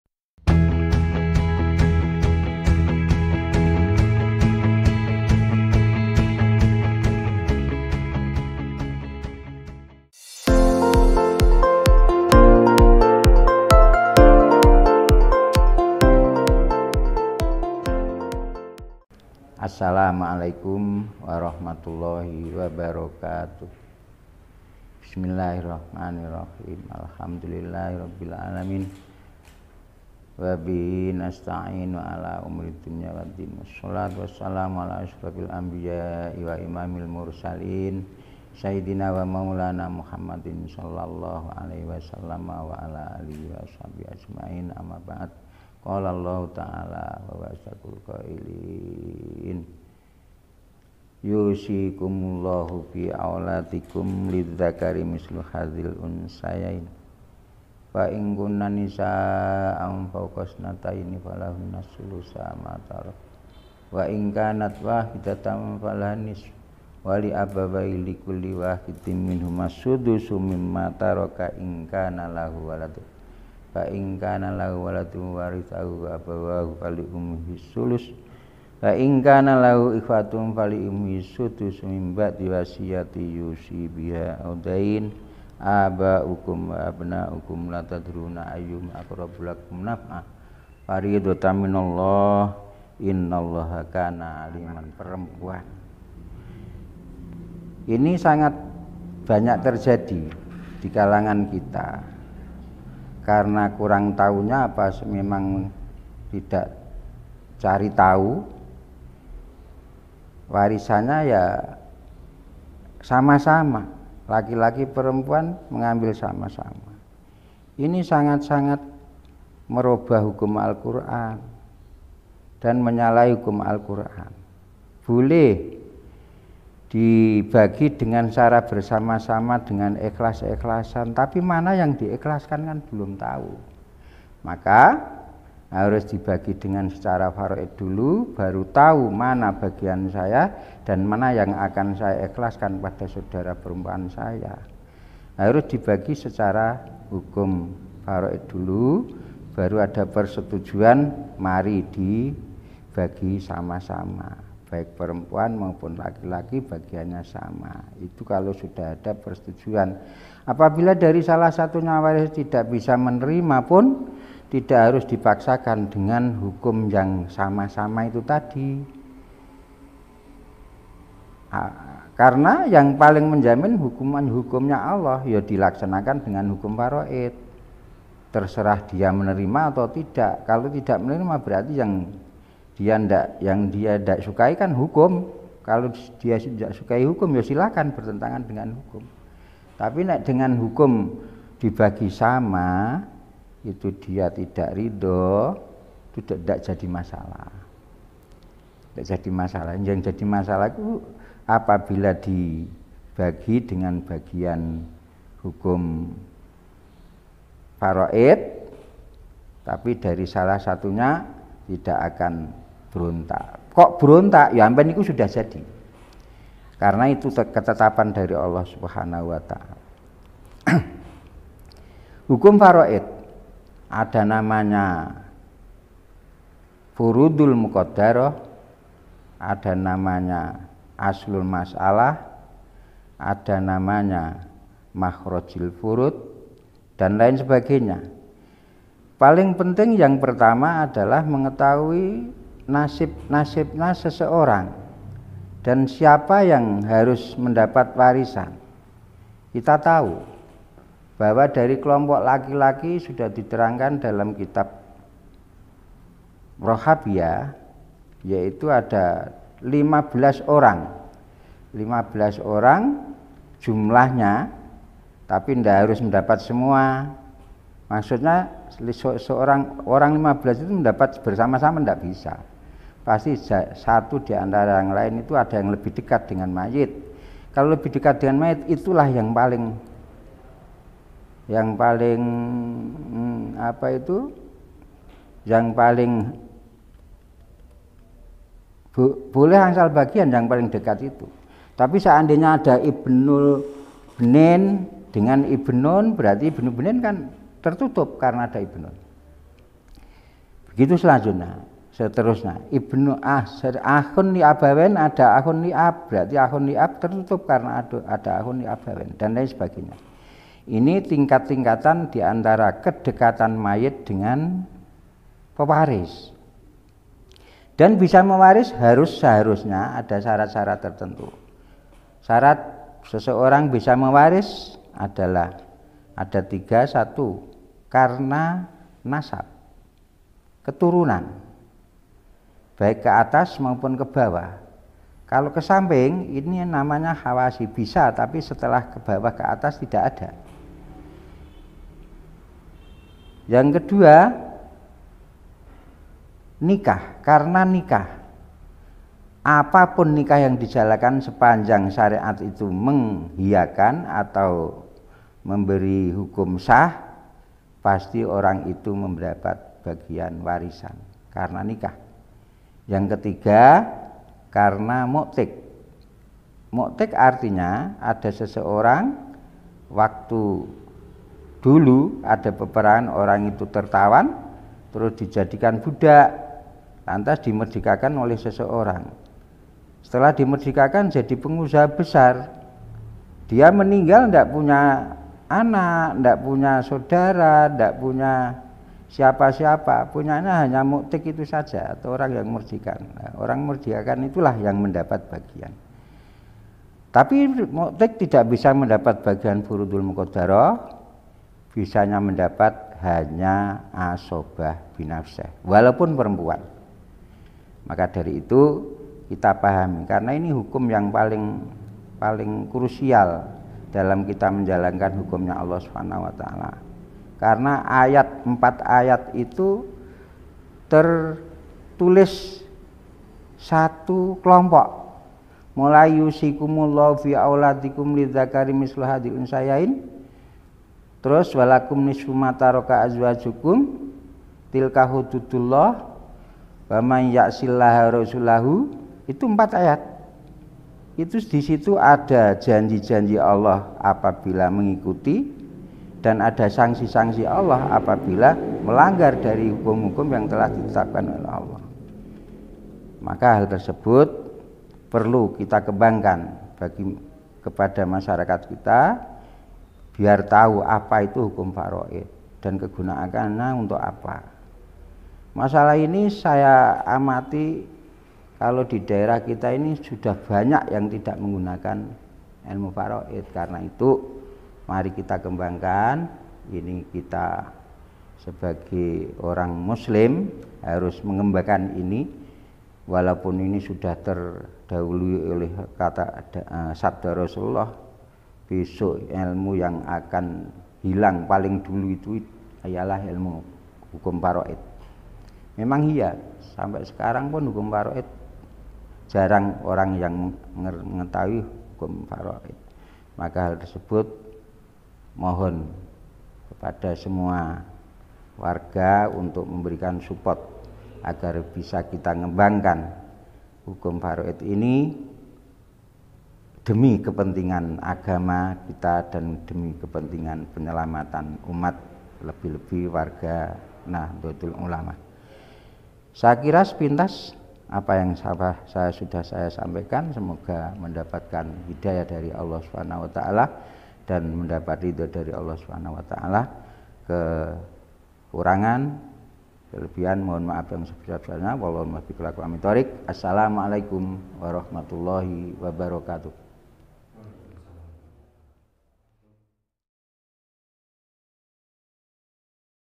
Pentingnya Ilmu Faraidh - Ilmu Waris Islam
Pentingnya ilmu faraidh / ilmu waris . ilmu yang semakin punah dan jarang di pelajari oleh santri di era millenial ini